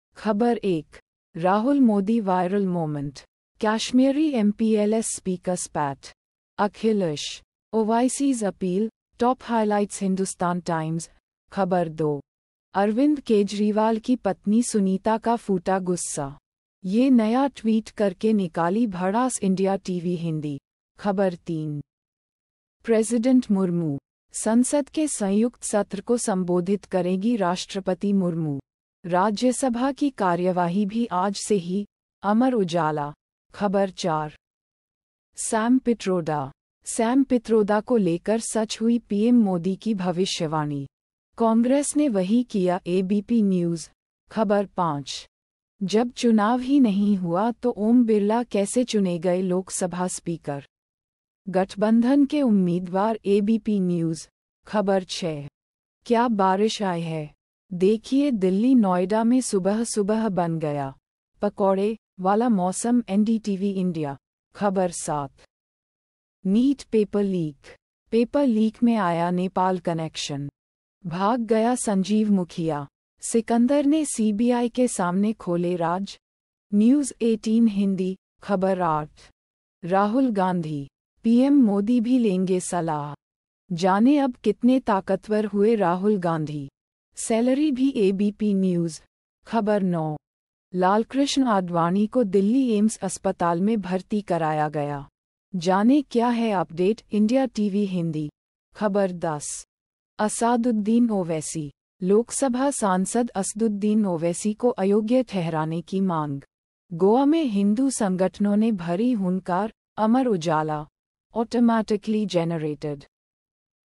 Hindi News (भारत)- 27/06/2024,10:11 AM GMT+5:30
ख़बर 1:Rahul-Modi Viral Moment, Kashmiri MP- LS Speaker Spat; Akhilesh, Owaisi’s Appeal | Top Highlights - Hindustan Times
ख़बर 2:अरविंद केजरीवाल की पत्नी सुनीता का फूटा गुस्सा, ये नया ट्वीट करके निकाली भड़ास - India TV Hindi
ख़बर 3:President Murmu: संसद के संयुक्त सत्र को संबोधित करेंगी राष्ट्रपति मुर्मू; राज्यसभा की कार्यवाही भी आज से ही - अमर उजाला
ख़बर 4:Sam Pitroda: सैम पित्रोदा को लेकर सच हुई पीएम मोदी की भविष्यवाणी, कांग्रेस ने वही किया - ABP न्यूज़
ख़बर 5:जब चुनाव ही नहीं हुआ तो ओम बिरला कैसे चुने गए लोकसभा स्पीकर, गठबंधन के उम्मीदवार - ABP न्यूज़
ख़बर 6:क्या बारिश आई है... देखिए दिल्ली-नोएडा में सुबह-सुबह बन गया 'पकौड़े' वाला मौसम - NDTV India
ख़बर 7:NEET Paper Leak: पेपर लीक में आया नेपाल कनेक्शन! भाग गया संजीव मुखिया, सिकंदर ने CBI के सामने खोले राज? - News18 हिंदी
ख़बर 8:Rahul Gandhi: पीएम मोदी भी लेंगे सलाह! जानें अब कितने ताकतवर हुए राहुल गांधी, सैलरी भी - ABP न्यूज़
ख़बर 9:लालकृष्ण आडवाणी को दिल्ली एम्स अस्पताल में भर्ती कराया गया, जानें क्या है अपडेट - India TV Hindi
ख़बर 10:Asaduddin Owaisi: लोकसभा सांसद असदुद्दीन ओवैसी को अयोग्य ठहराने की मांग, गोवा में हिंदू संगठनों ने भरी हुंकार - अमर उजाला

Automatically Generated...